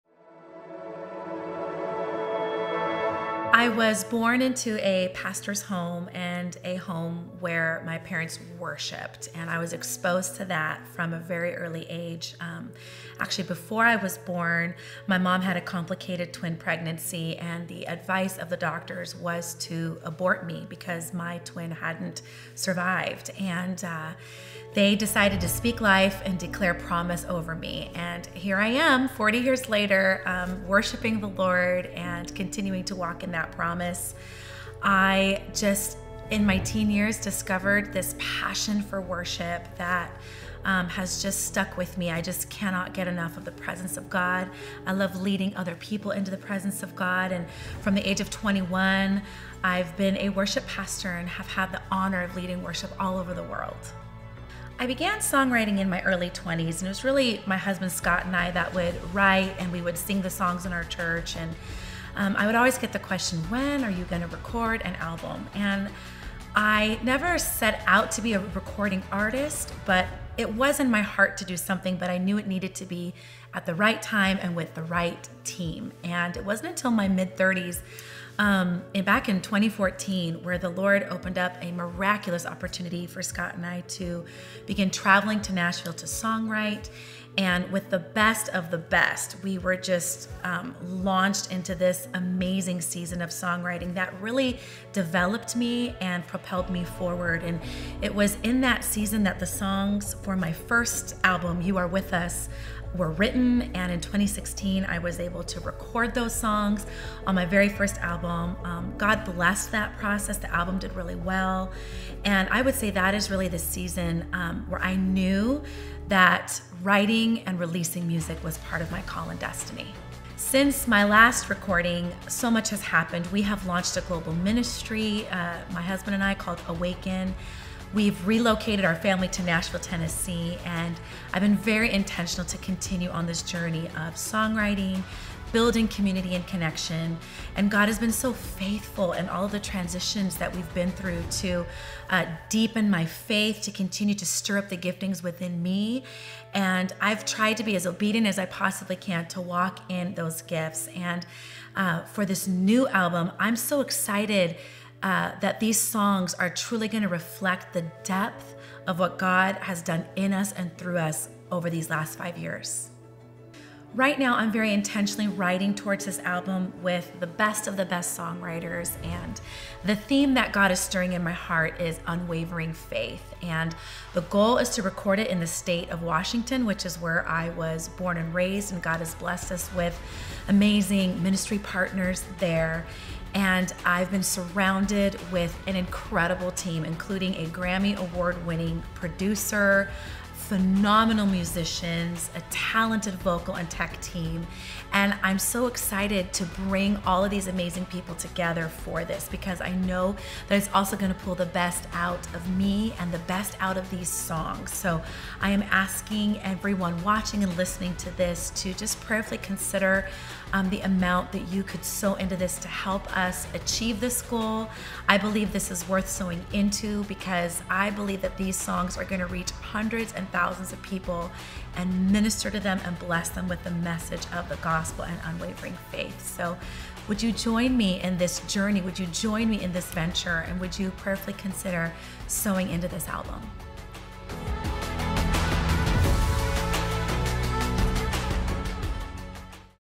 Lydia Ingegneri | Live Album Recording Promo!
Hey everyone! I am so excited to announce that I will be recording a live album in Olympia, WA this June! I am so excited and expectant for what God is going to do with these songs.